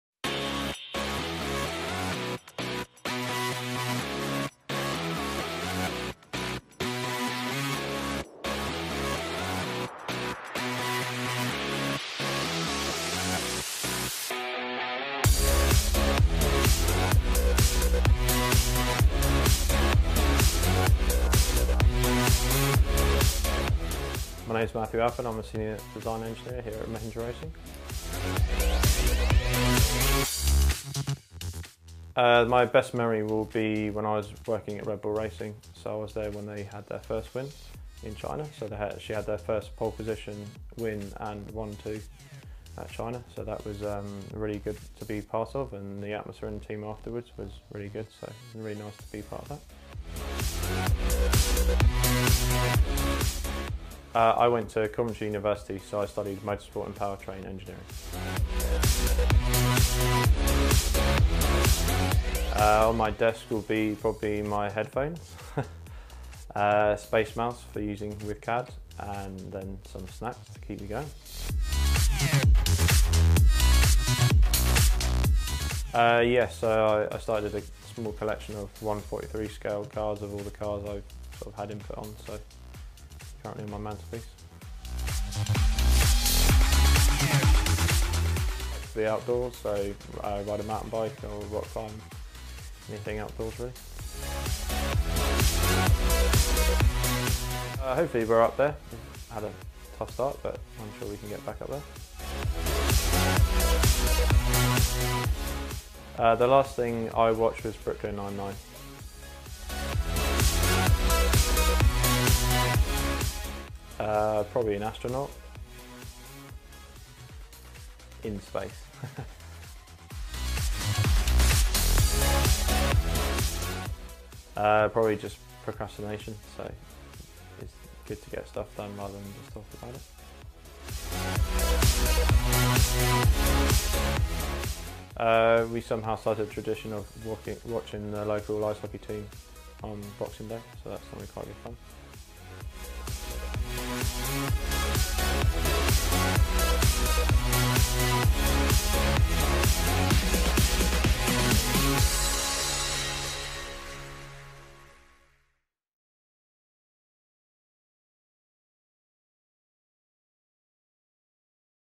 Meet The Team with Matthew Alford - Senior Design Engineer | Mahindra Racing
In this series, we introduce you to members of the Mahindra Racing team! Get to know the people behind the squad, as we give you the lowdown on the people behind our team. In this episode, meet Matthew Alford, a Senior Design Engineer at the team.